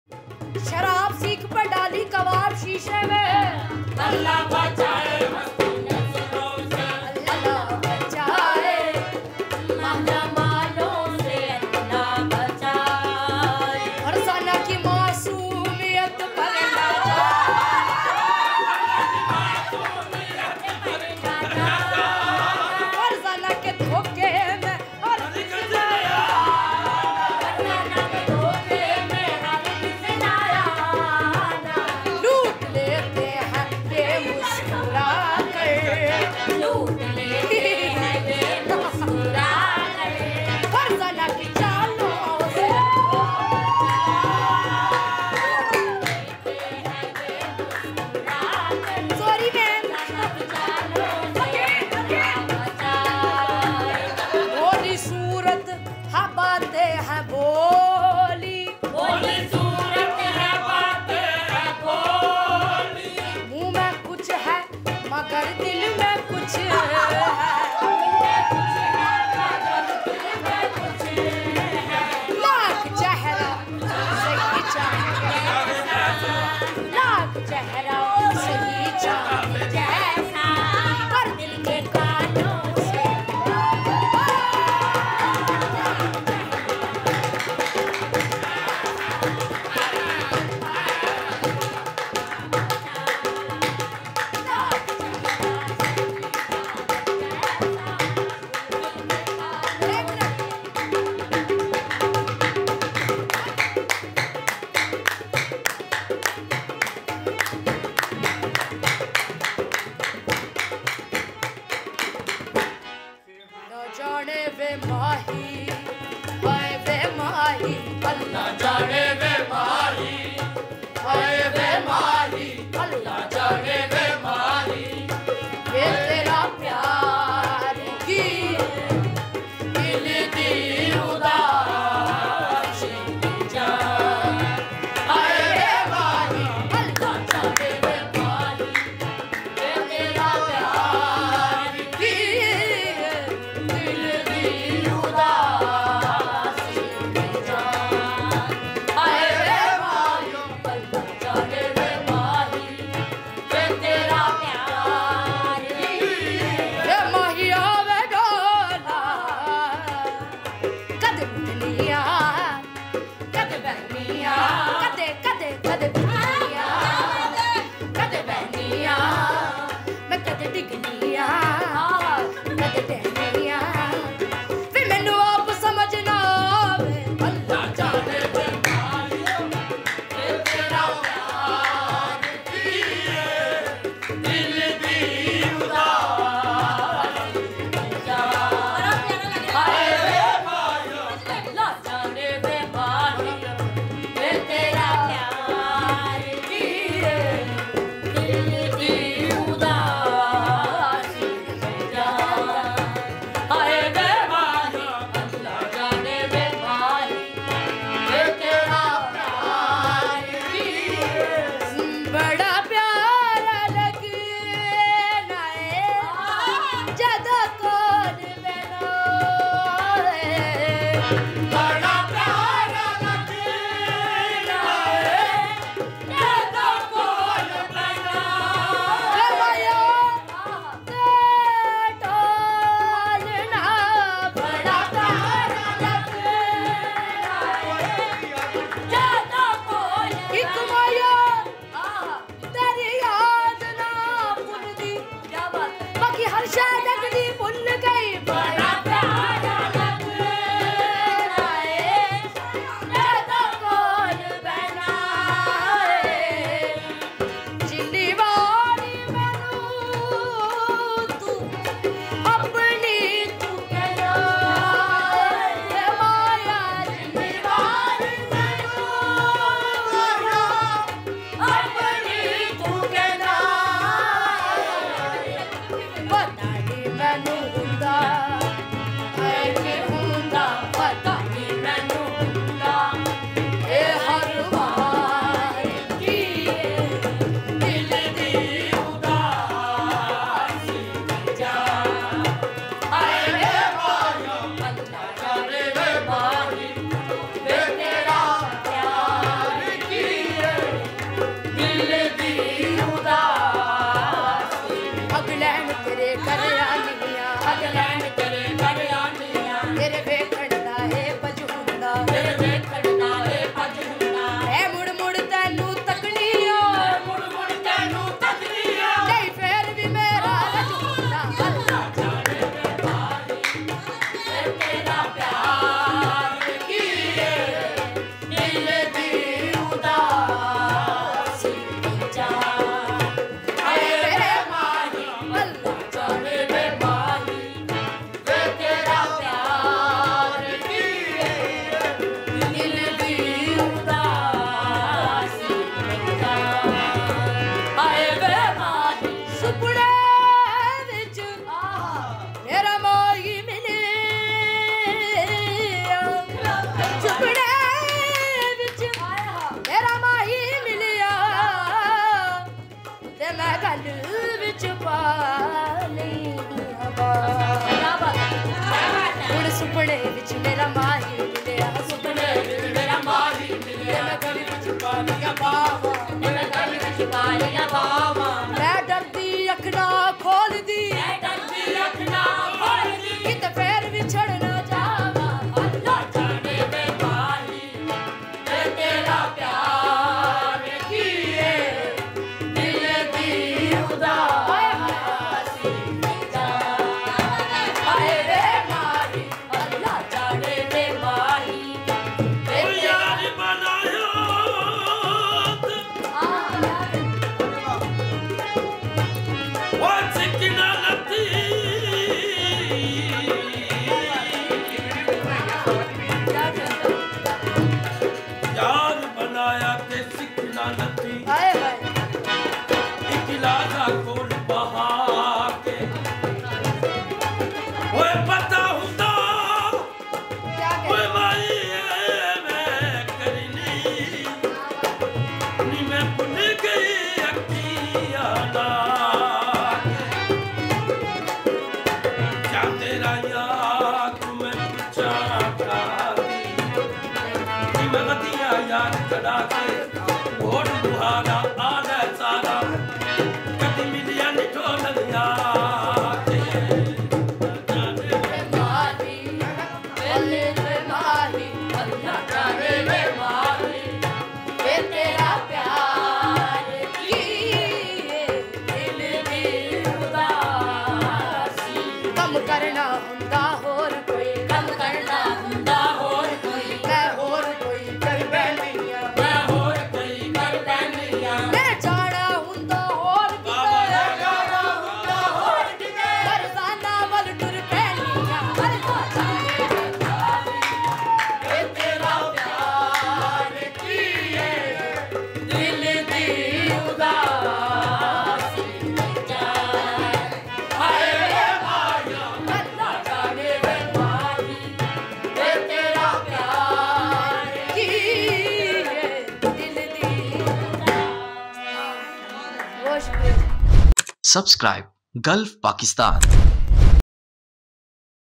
SIDRA QAWAL AUR HUMNAWA GROUP NY NOORAN SISTERS KI YAD TAZA KAR DI | GULF QWALI NIGHT SPECIAL
Program: GULF SPECIAL QAWAL PROGRAM
Program Host: AYESHA TEHSEEN
PROGRAM GUEST: NADEEM JAMEEL
SPECIAL THANKS : AHAD KHAN
Program Director:  MUHAMMAD SAJID ALI SAJID
DOP: SYEDA QUDSIA
Edit: ANOOSH BHATTI
Produce By: Gulf Pakistan Media Network
Present By: Gulf Pakistan Media Network